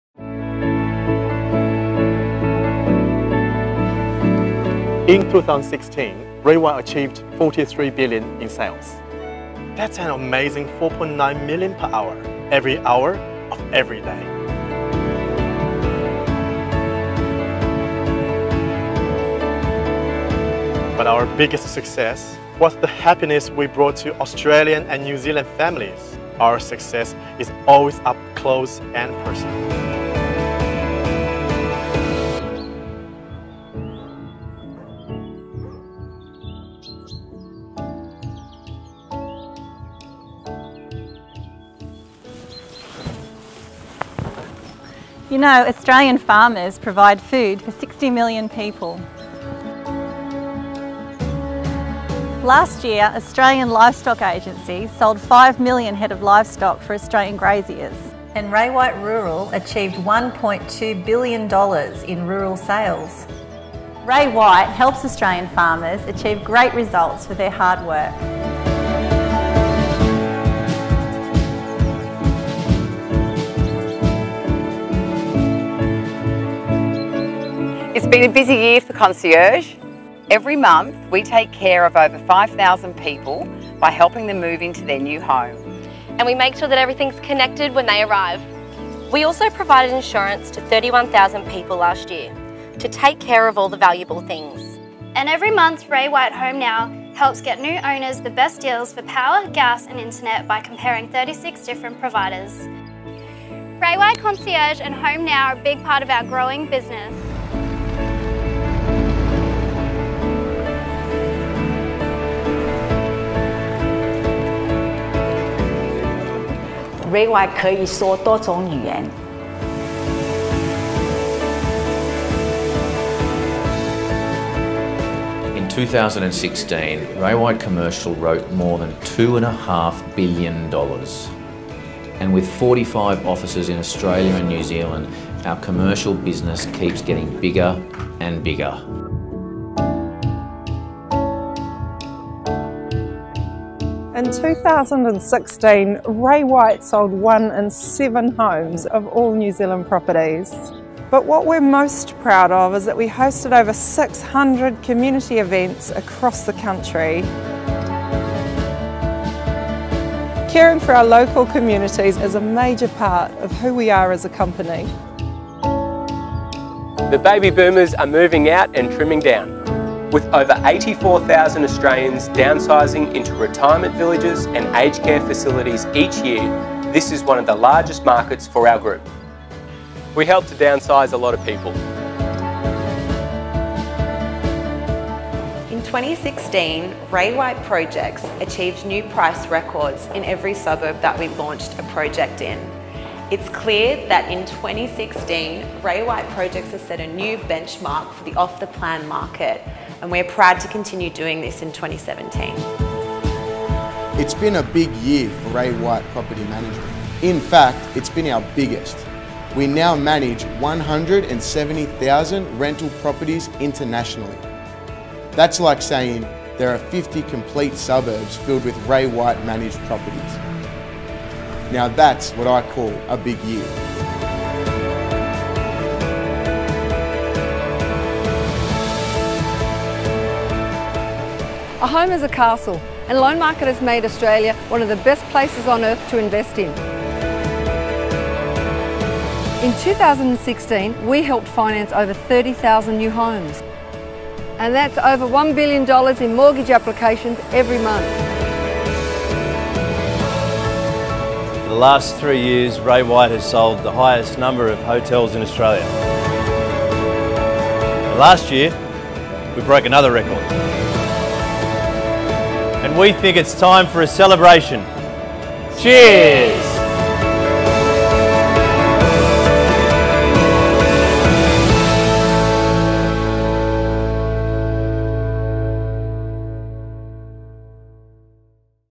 Ray White Group Performance 2016
$43billion in residential sales, numerous group records, great customer feedback and thousands of community events.

We look back at the last financial year and could not be prouder of the strength and breadth of our family.